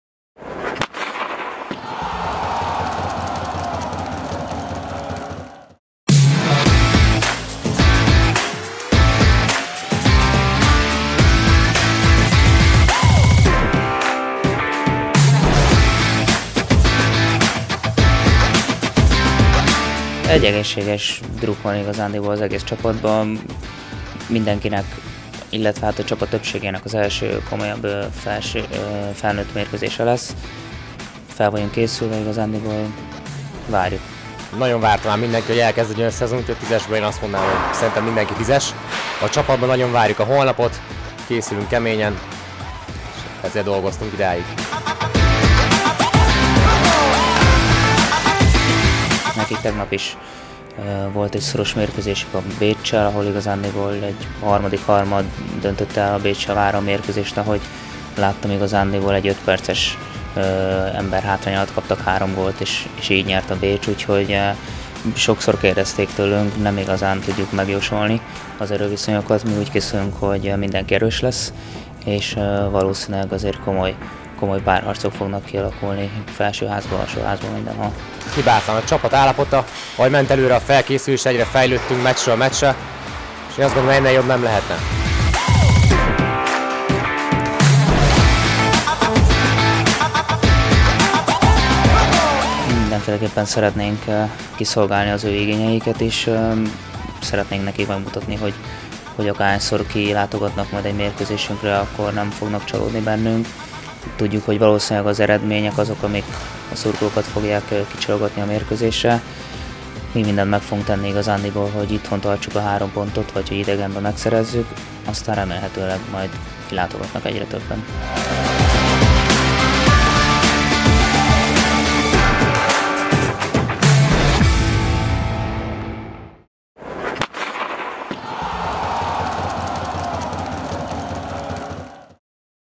Kezdődik a szezon!
Indul az Erste Liga 2018/2019-es szezonja. A Fehérvári Titánok a Hokiklub Budapest ellen kezdi a bajnoki mérkőzések sorát. Kiss Dávid vezetőedző és Gyenes Dávid, az együttes kapusa beszélt a várakozásokról.